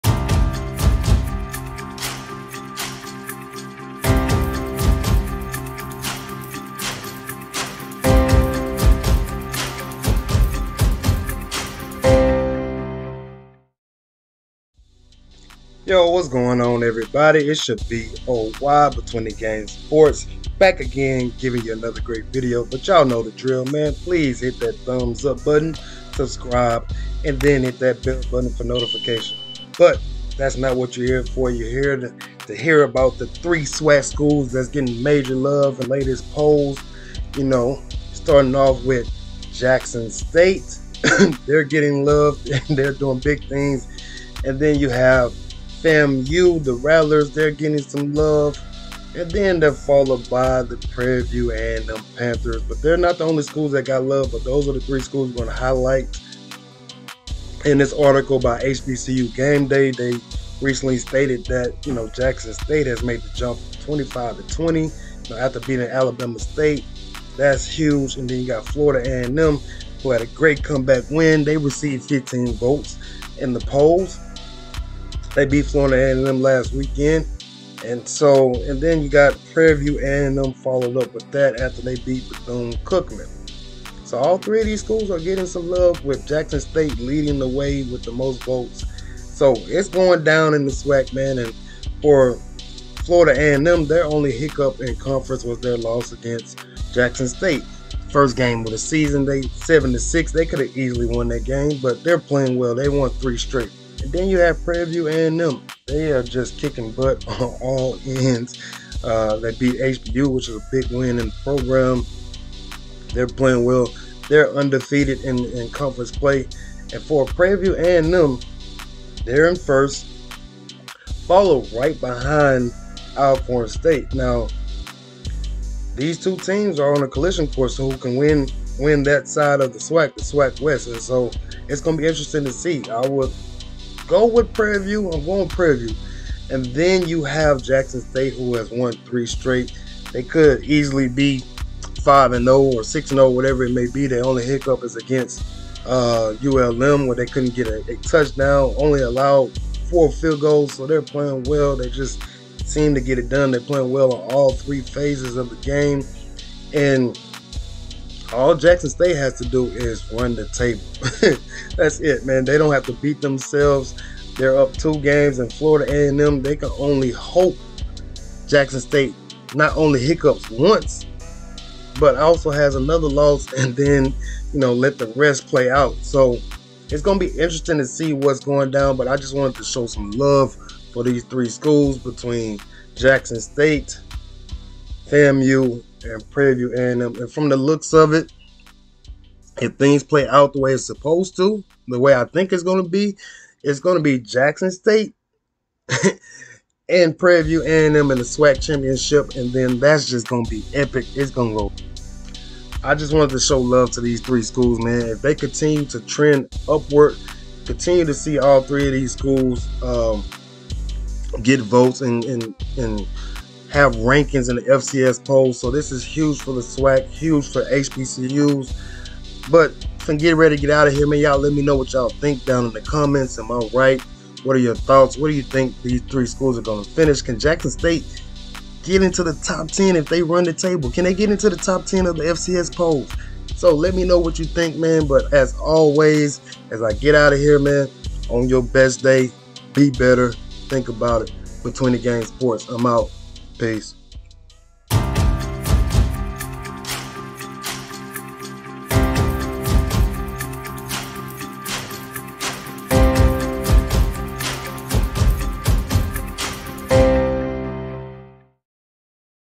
SWAC SCHOOLS: ON THE RISE IN FCS POLLS!! JACKSON STATE RANKED 20TH
LATEST FCS POLLS ARE SHOWING LOVE TO 3 SWAC SCHOOLS, JACKSON STATE, FLORIDA A&M, AND PRARIE VIEW A&M. MUCH RESPECT TO ALL 3 OF THESE SCHOOLS. LET'S HOPE THEY ALL CONTINUE TO CLIMB THE RANKING BOARDS. LEAVE US YOUR THOUGHT DOWN IN THE COMMENTS.